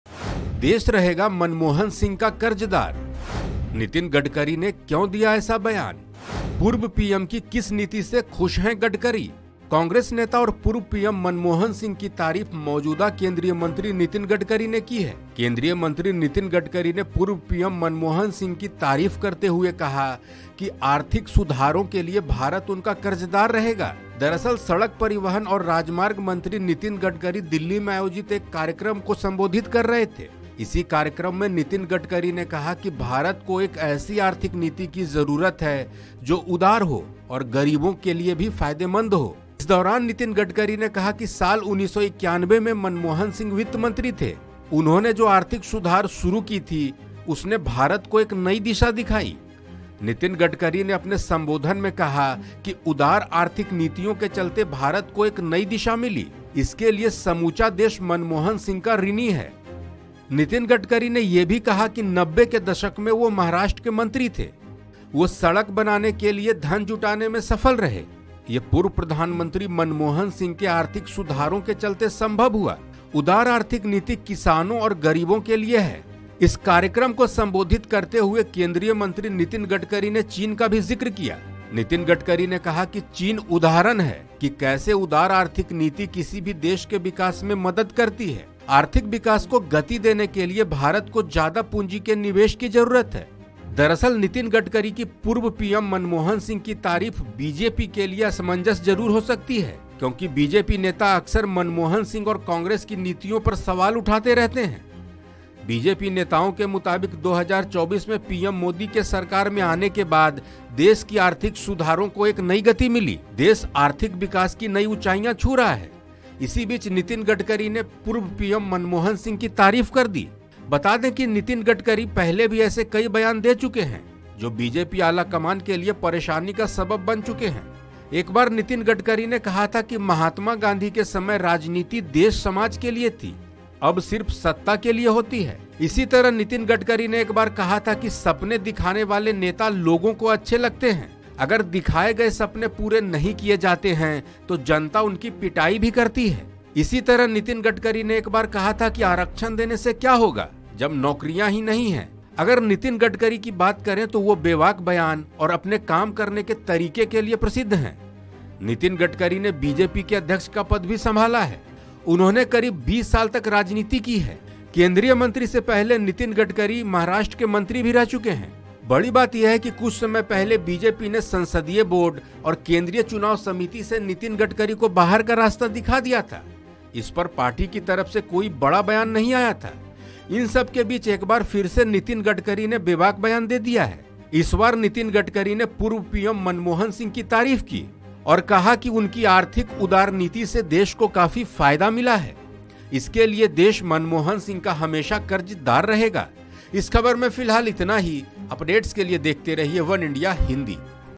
Union Minister Nitin Gadkari ने की Former PM Manmohan Singh की तारीफ | वनइंडिया हिंदी *News
कांग्रेस नेता (Congress leader) और पूर्व पीएम मनमोहन सिंह (Manmohan Singh) की तारीफ मौजूदा केंद्रीय मंत्री नितिन गडकरी (Nitin Gadkari) ने की है. केंद्रीय मंत्री नितिन गडकरी ने पूर्व पीएम मनमोहन सिंह (Nitin Gadkari praised Manmohan Singh) की तारीफ करते हुए कहा कि आर्थिक सुधारों (economic reforms) के लिए भारत उनका कर्जदार रहेगा.